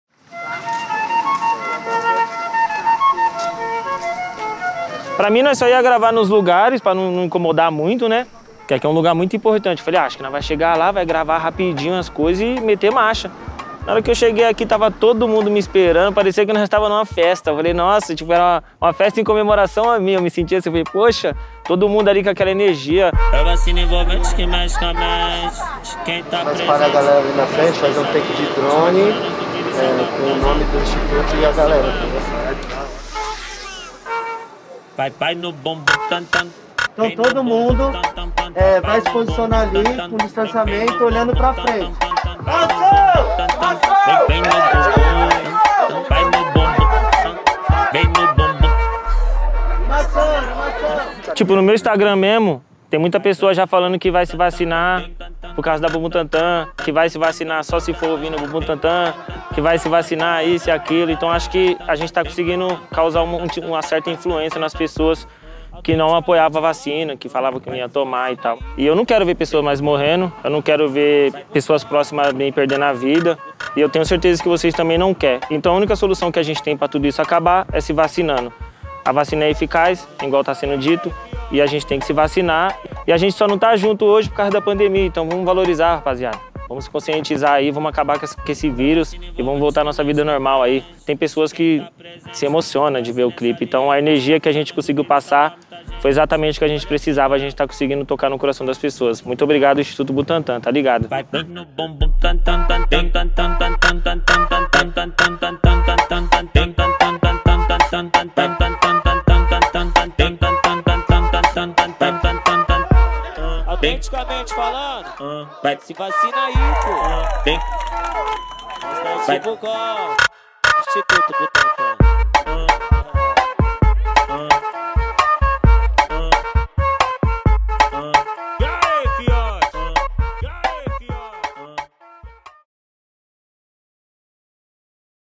Os bastidores do clipe do Mc Fioti no Instituto Butantan
▶  Instituto Butantan - A serviço da Vida